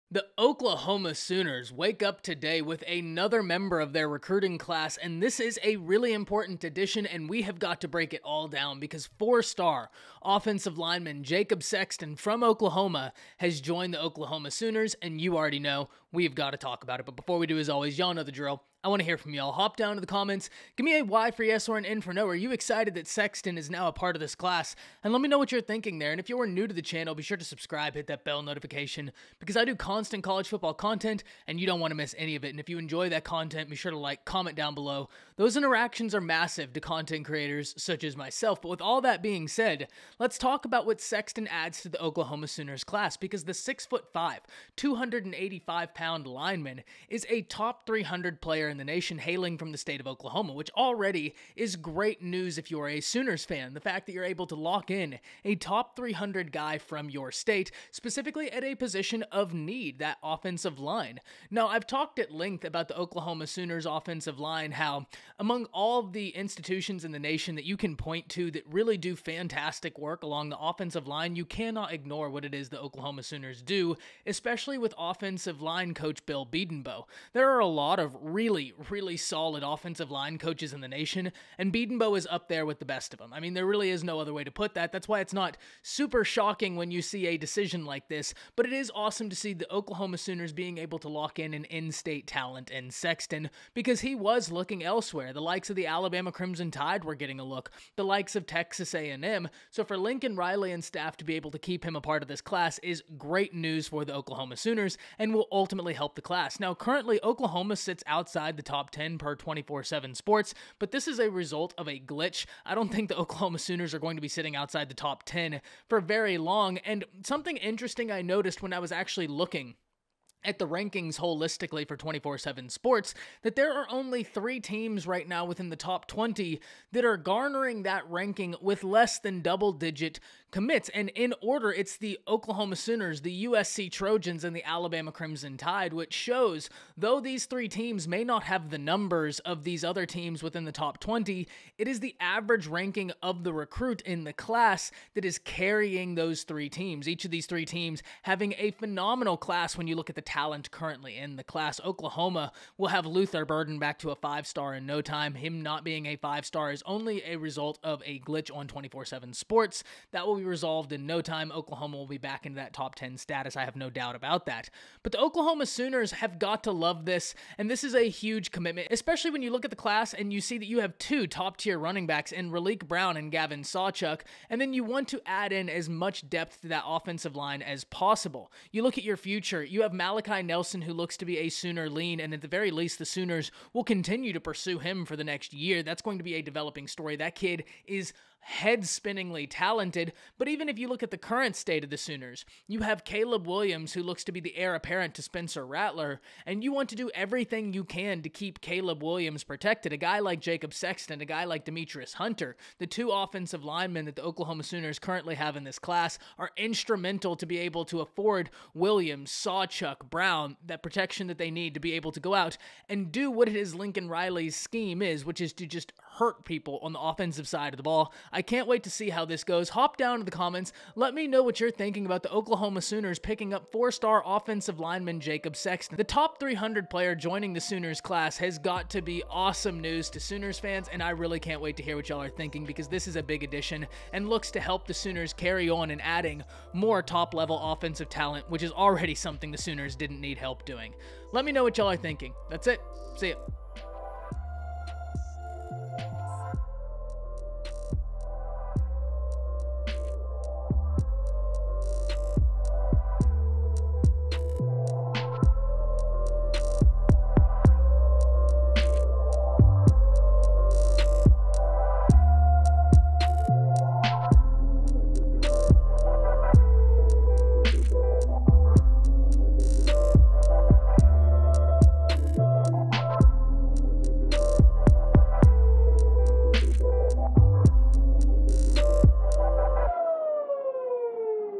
The Oklahoma Sooners get commitment from TALENTED 2022 OL Jacob Sexton! What this means for OU!
The Oklahoma Sooners added a really important piece to their class yesterday as top 300 OL Jacob Sexton committed to the Sooners. What does this mean for Lincoln Riley and Oklahoma Football?